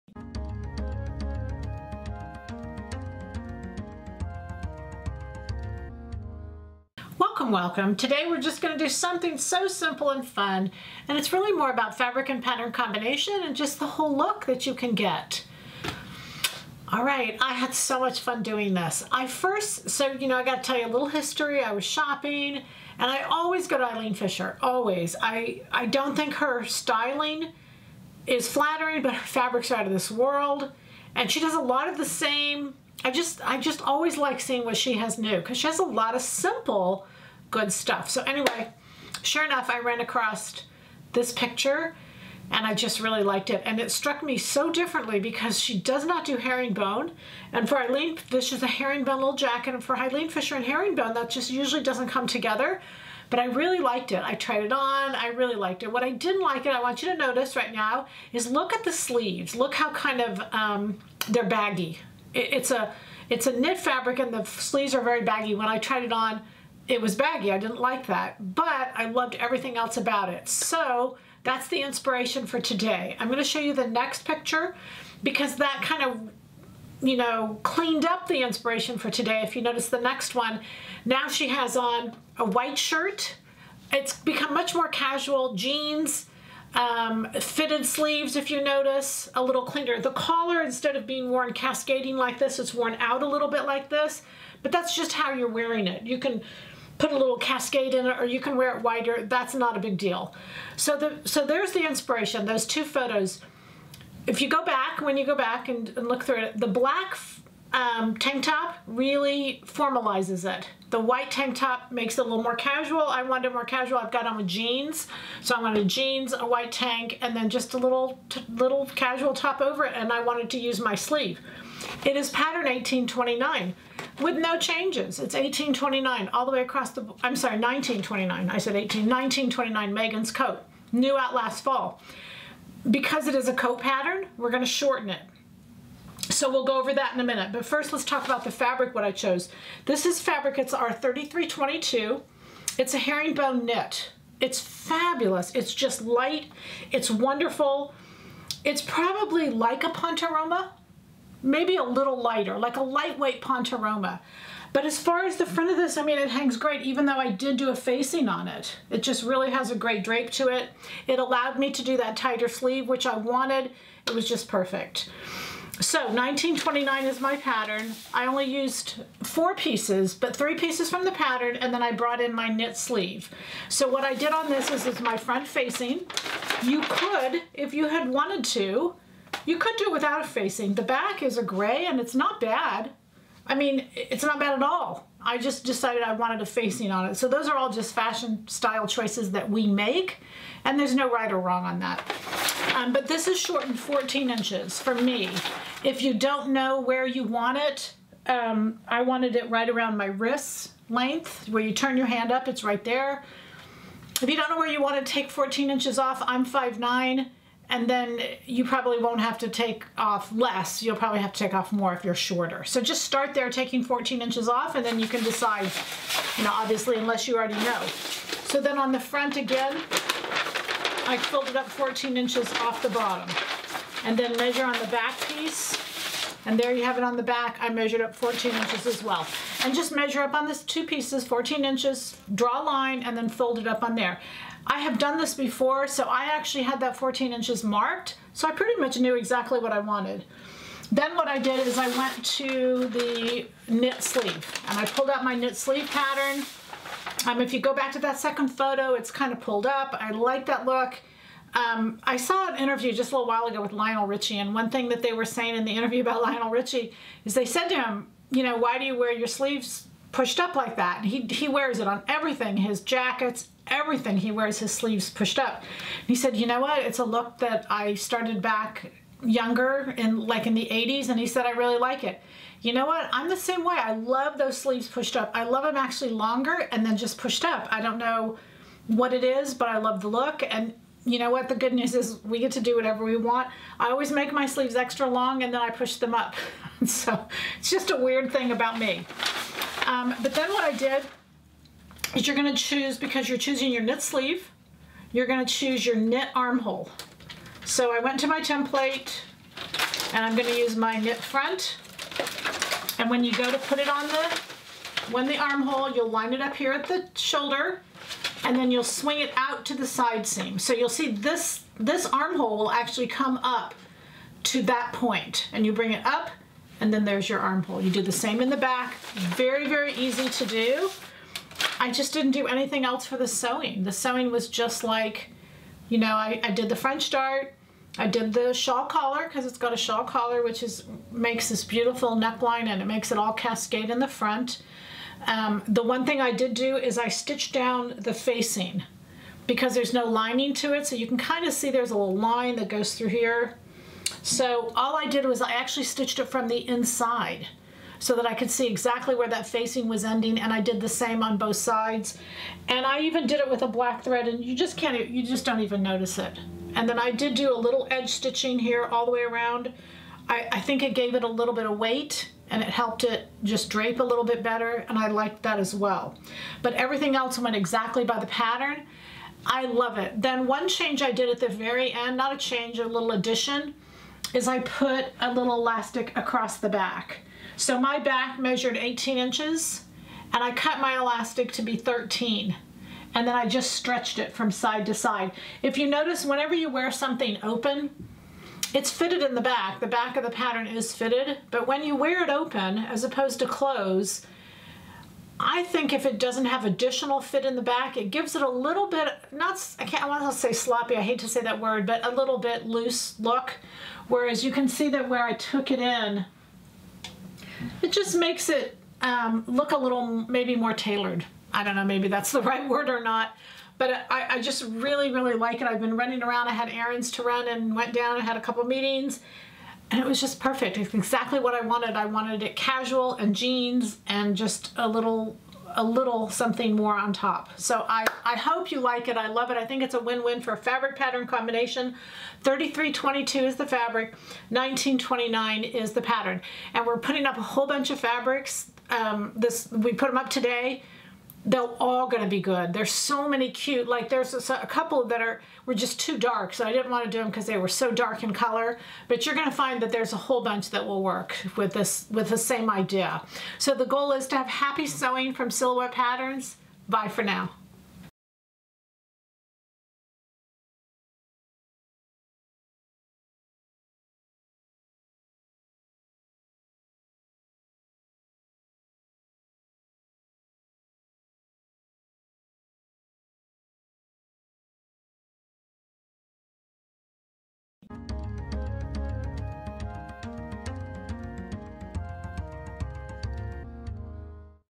Let's Sew - Episode 164 - EF Meets Herringbone
After finding a great piece from Eileen Fisher we go over some great ways to make it your own.